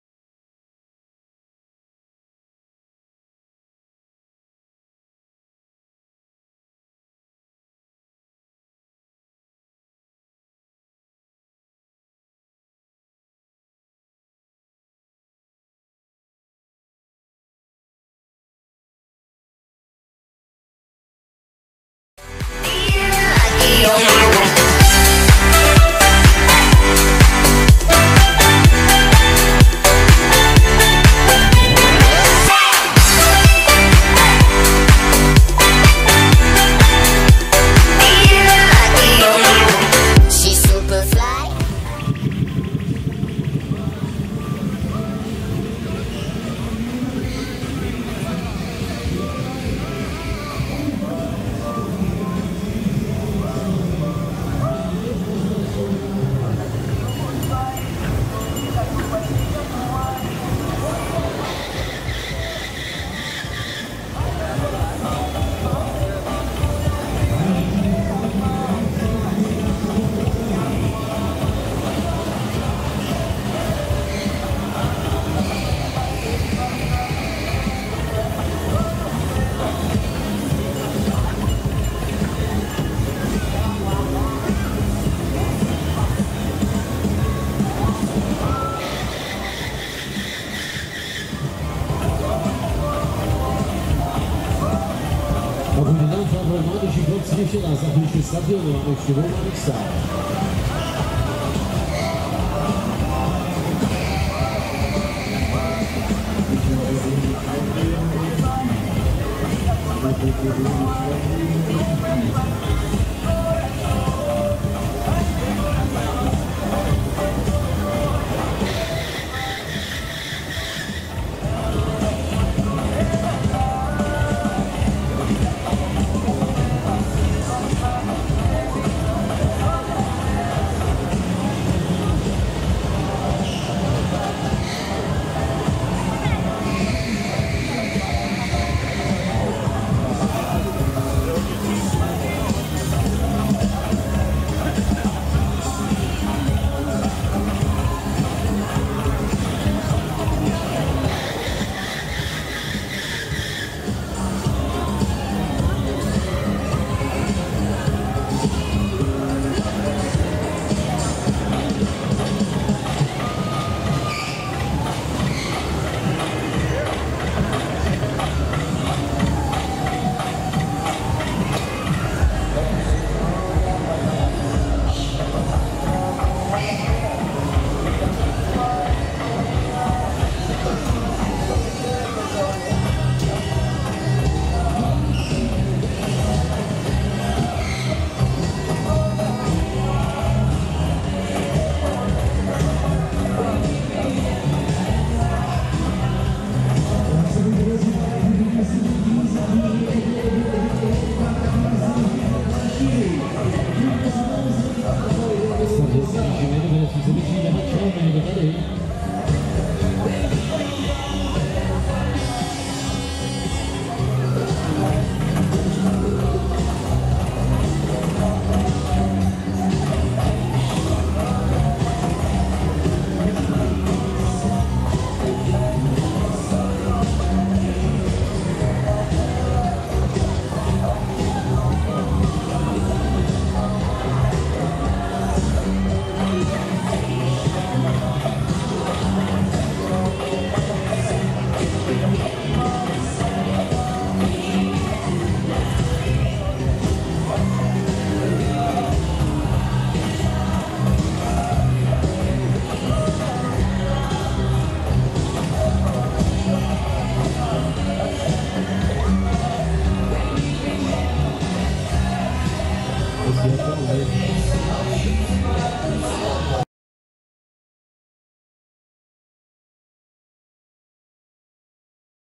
Heřmanoměstecká pouť 2019-atrakce Break Dance O
Datum konání  od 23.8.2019 do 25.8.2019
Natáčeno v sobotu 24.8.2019
Provozovatel Jiří Maleček Lunapark Maleček